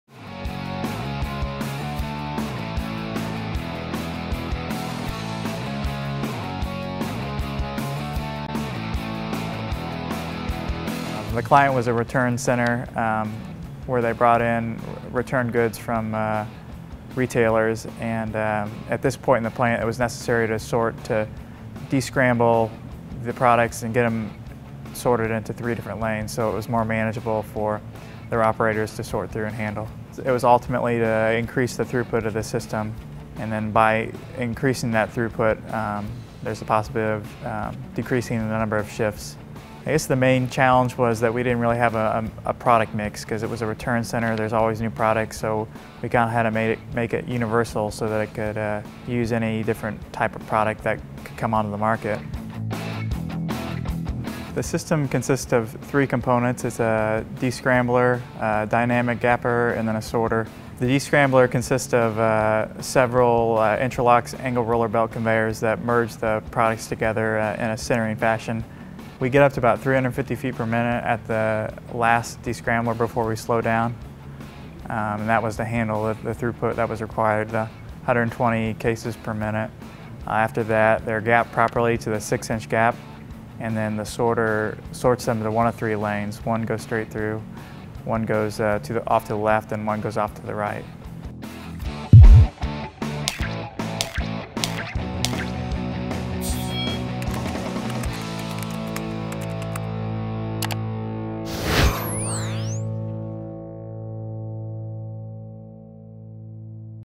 ARB Descrambler & Sorter by Bastian Solutions
This custom conveyor system featured activated roller belt (ARB) technology to singulate, gap, and sort a variety of products for a returns center in Indianapolis.